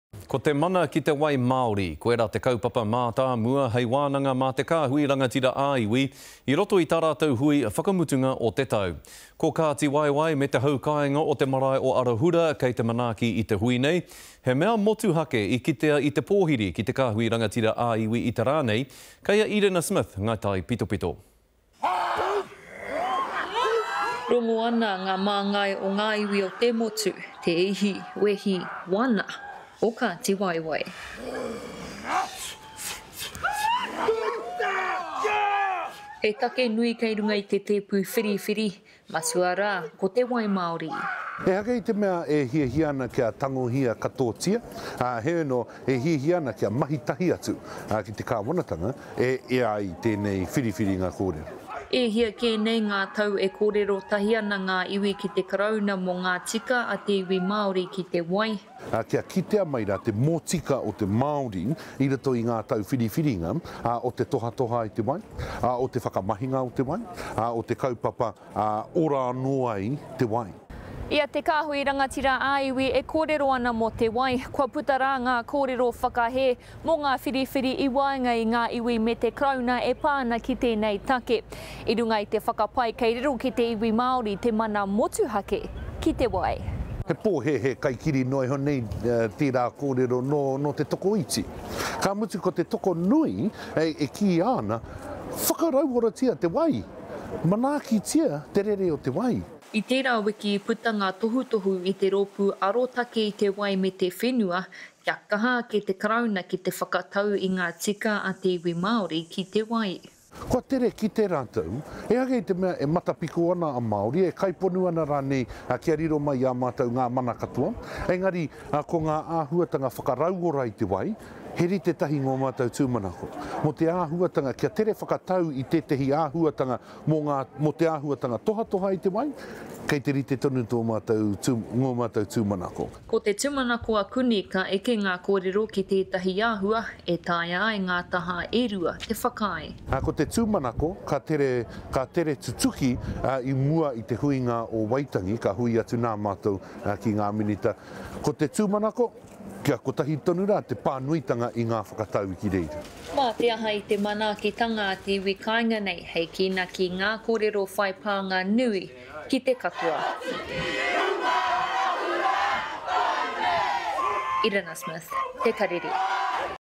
Iwi leaders meet over freshwater rights
Water rights is top of the agenda at the last Iwi Leaders Forum hui for the year. The event is being held at Arahura Marae in Hokitika and discussions on how to progress negotiations with the Crown on Māori interests in freshwater.